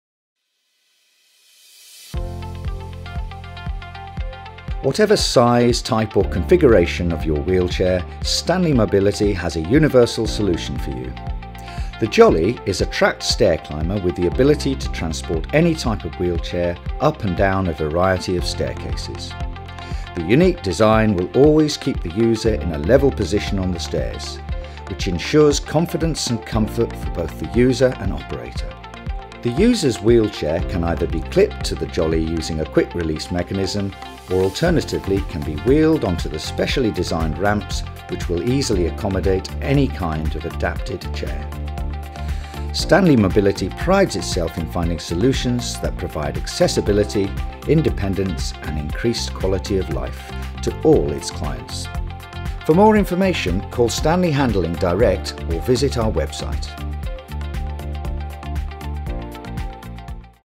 Stanley Presents The Jolly Wheelchair Stairclimber
The Jolly machine is a wheelchair stairclimber provides the option for users own wheelchair to be secured onto its metal frame. It then uses a powered track drive to transport the user up and down stairs with comfort and safety.